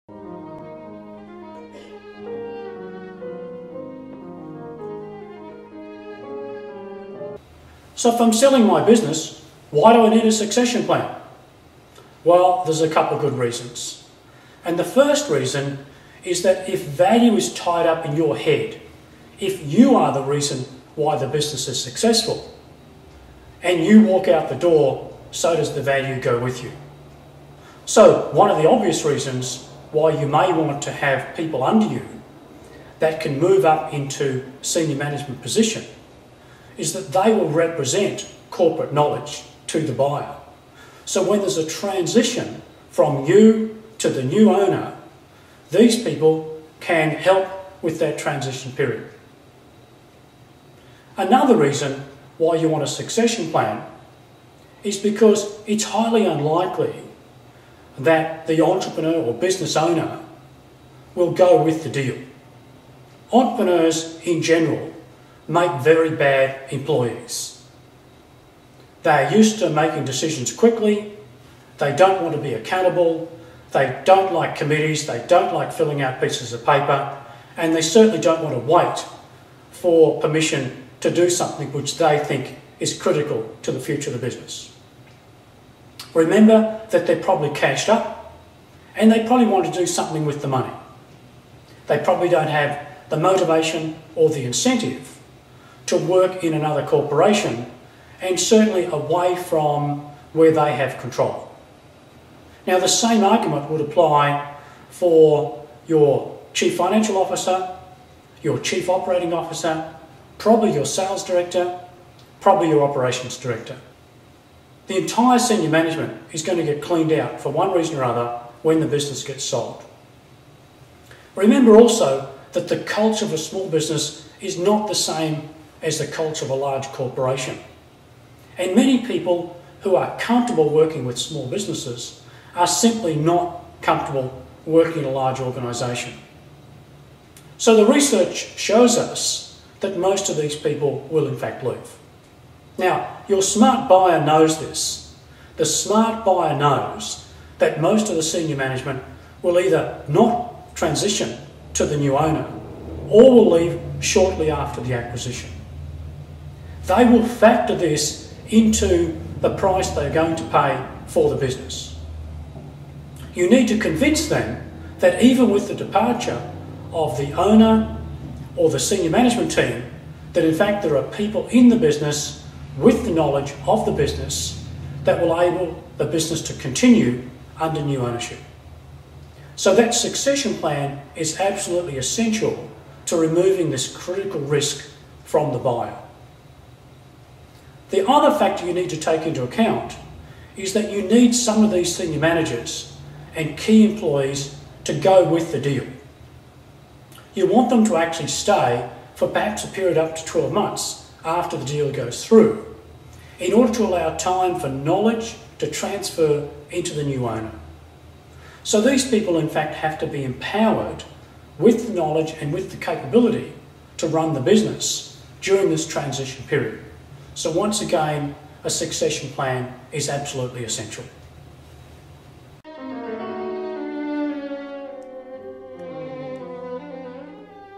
Why do I need a succession plan?
Selling a Business 20:
You can increase the value on sale by setting up a succession plan which will assure the buyer that corporate knowledge will be transferred with the business.